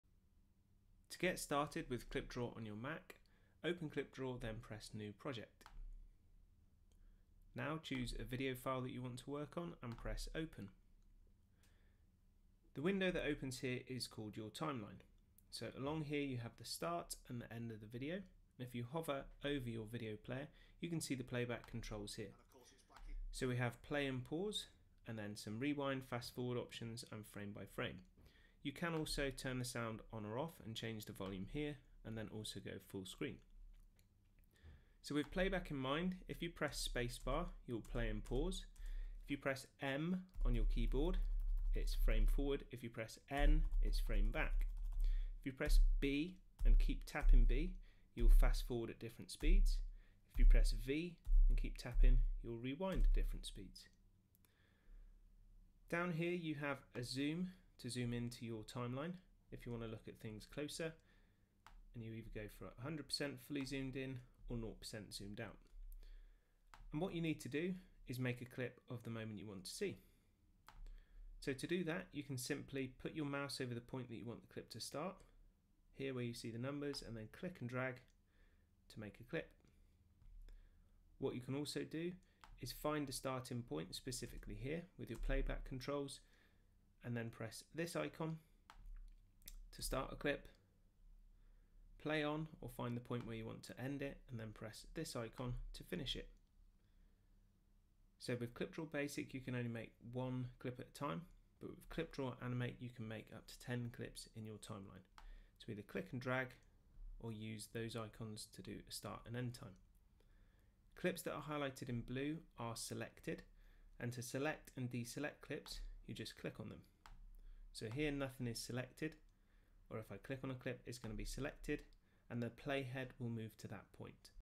KlipDraw - 1 - Make and edit Klips
This video will show you how to make and edit Klip lengths on the KlipDraw Mac timeline.